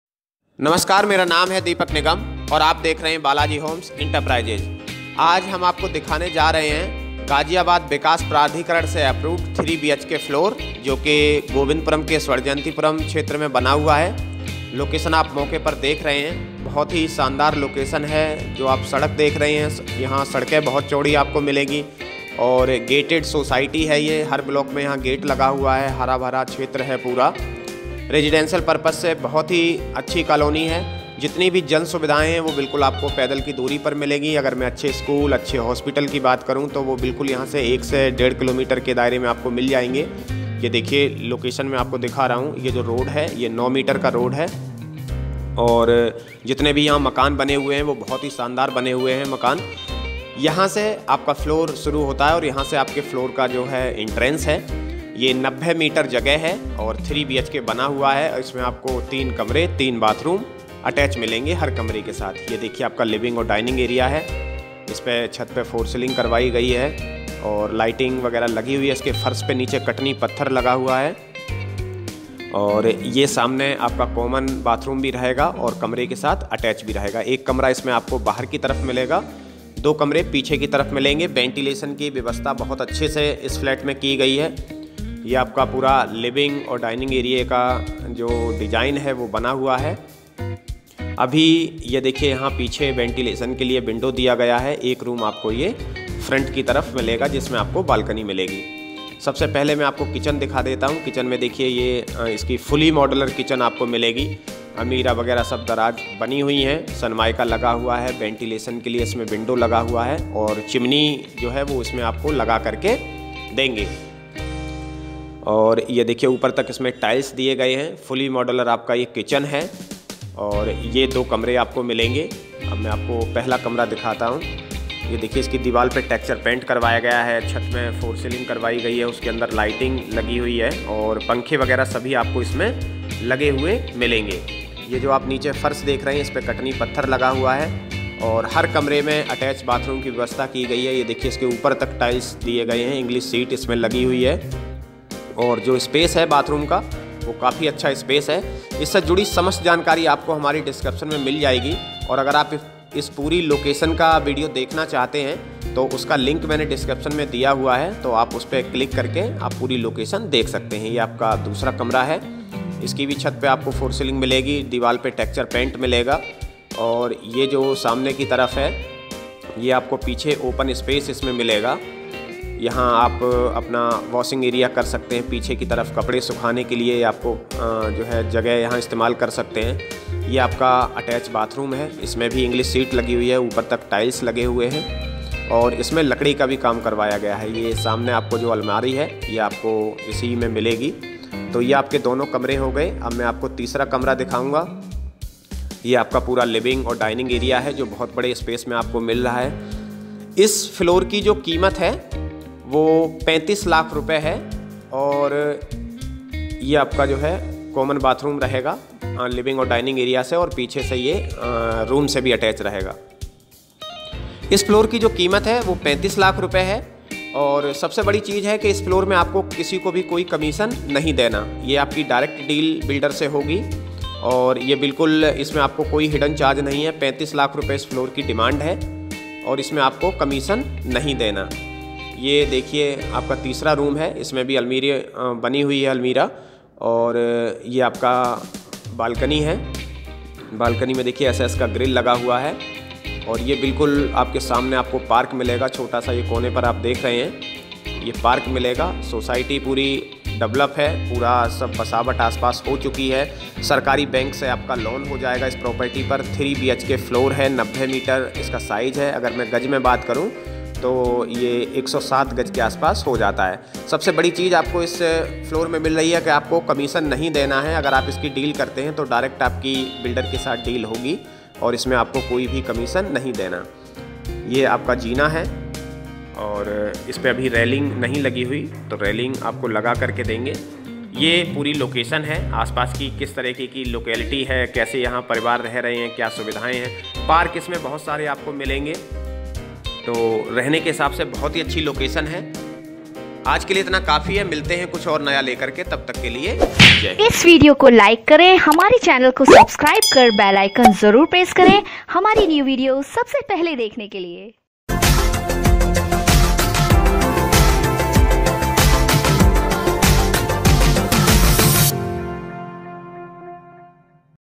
3 BHK Floor | G D A Approved | No Brokerage | Direct Deal | Govindpuram
3 BHK Floor | G D A Approved | No Brokerage | Direct Deal | Govindpuram |

वीडियो के बारे में :
इस वीडियो में आपको  90 मीटर  जी डी ए फ्लैट दिखाया गया है।
इस  फ्लैट की कीमत  35 लाख रूपये है।
कॉलोनी का नाम - स्वर्णजयंती पुरम (गाजियाबाद विकास प्राधिकरण से स्वीकृत)
प्लाट साइज - 90,112,150,200 मीटर
कीमत प्लाट - 90 मीटर (48 लाख) 112 मीटर (60 लाख) 150 मीटर (70 लाख) 200 मीटर (80 लाख)
कीमत फ्लैट - 90 मीटर (35 लाख) 112 मीटर (42 लाख) 150 मीटर (55 लाख) 200 मीटर (70 लाख)

रोड साइज - 80 फ़ीट का सबसे बड़ा मेन रोड
60 फ़ीट रोड ,40 फ़ीट रोड,
सबसे छोटा 30 फ़ीट रोड
सुविधायें :-
पार्क
सीवर लाइन
गेट बंद कॉलोनी
हरा भरा वातावरण,चौड़ी सड़कें
24 घंटे बिजली पानी
स्कूल,बाजार,बैंक पैदल की दूरी पर
पेट्रोल पम्प ,सी एन जी पम्प  पैदल की दूरी पर